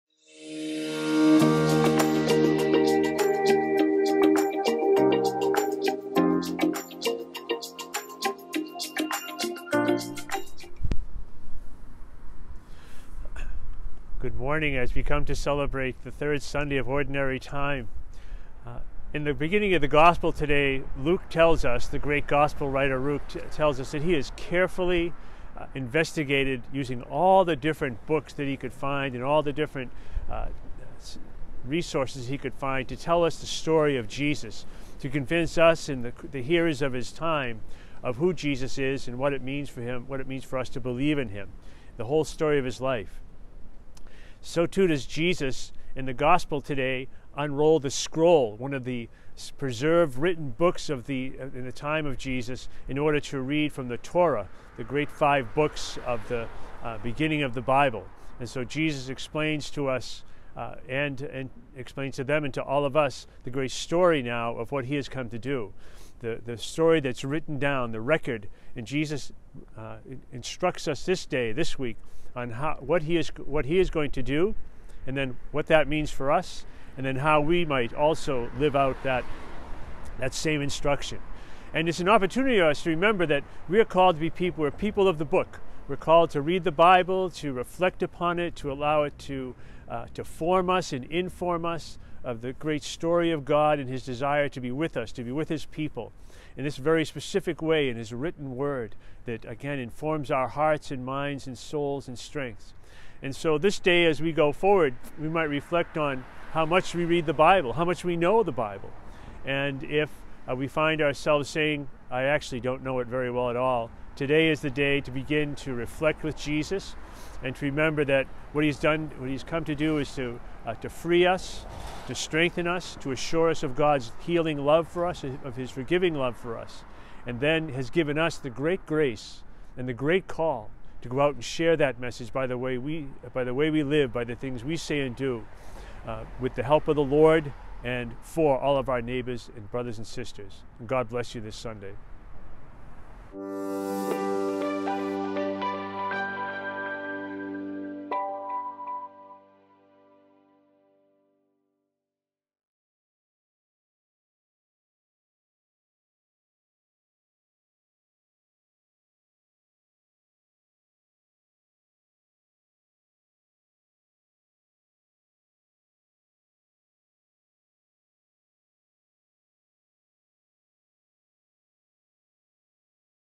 FM's Message | Sunday, January 23rd, 2022
Fr. Murray (FM) reflects on this Sunday’s Gospel on location at the Salem Public Library in Salem, Massachusetts.